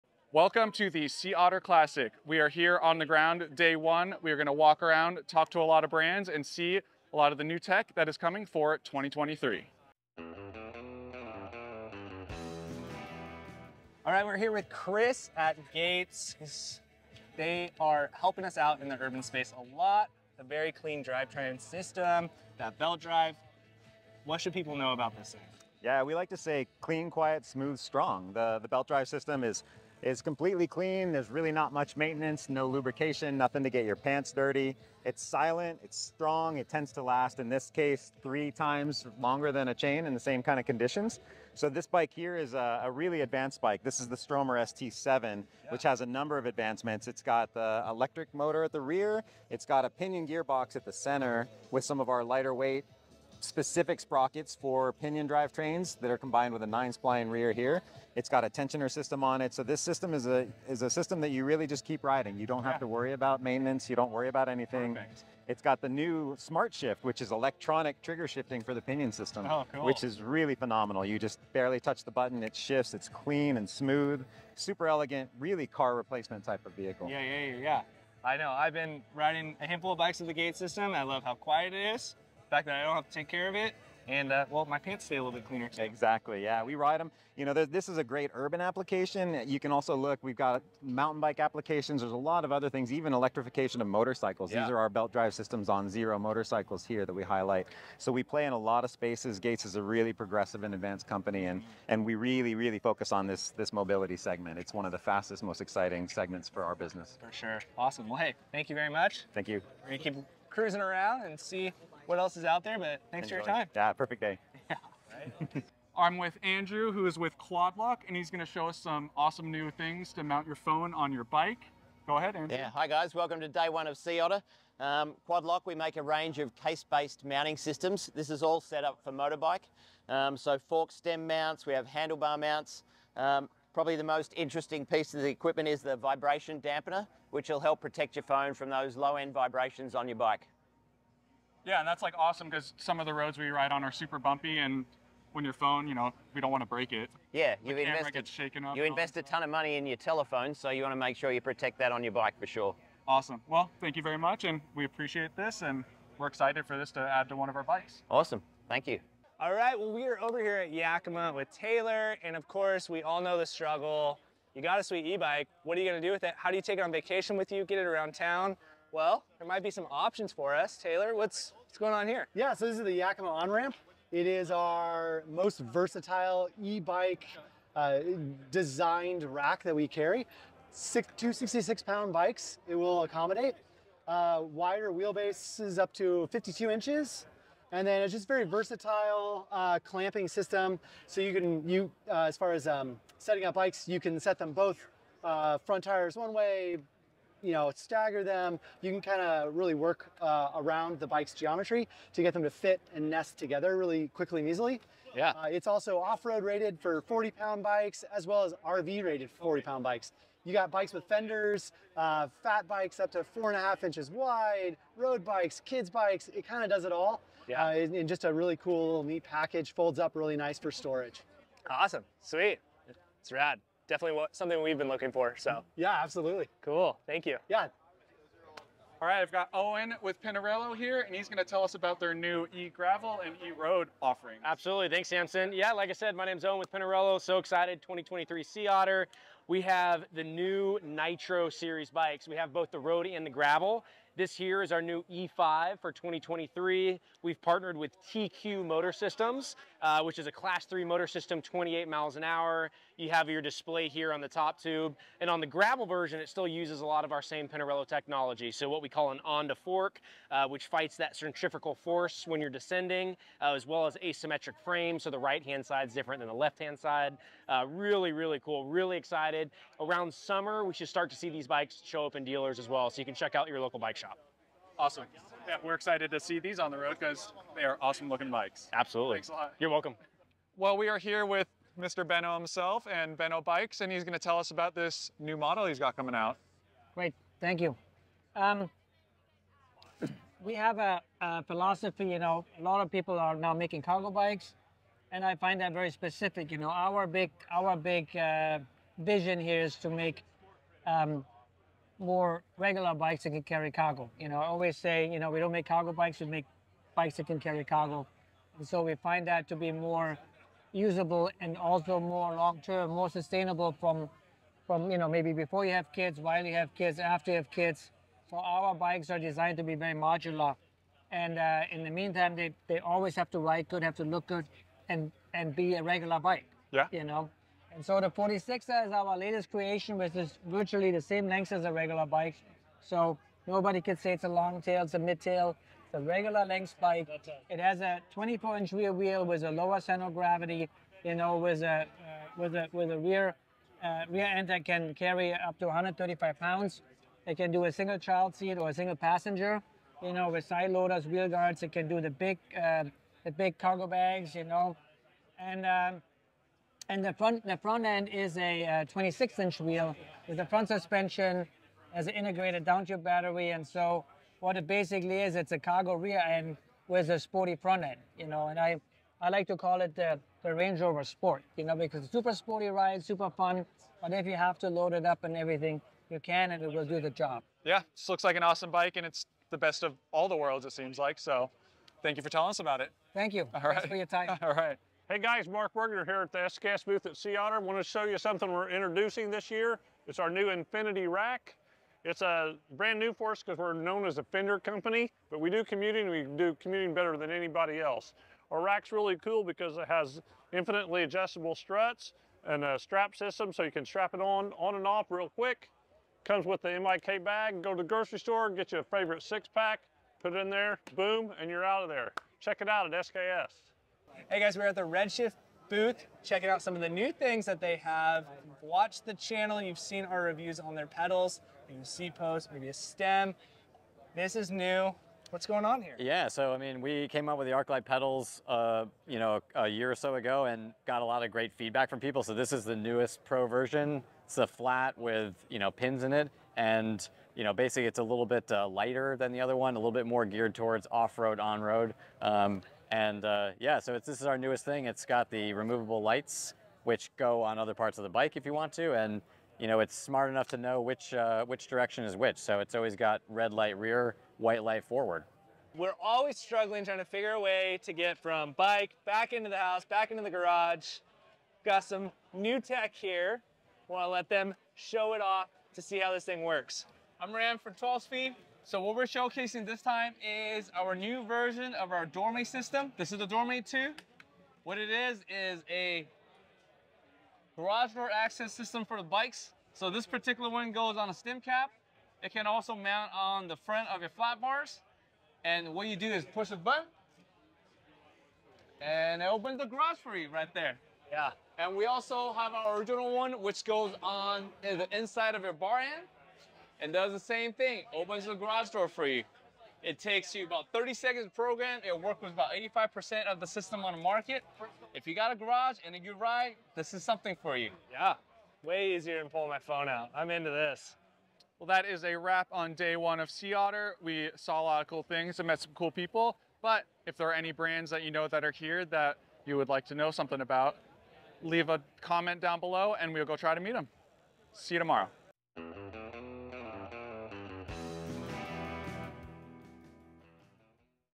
Sea Otter Classic Day 1 Recap! Cool new ebike gear and tech .
We are on the floor at Sea Otter Classic talking to brands and seeing what cool new gear and tech is out there.

If there is anyone you would like to see, send us a comment and we will try and check them out!

00:00 Intro
00:15 Gates Carbon Belt Drive
01:56 Quad Lock
02:39 Yakima Racks
03:49 Pinarello Bikes
04:40 Benno Bikes
06:25 SKS Racks
06:54 Redshift
07:36 12 Speed Products
08:31 Outro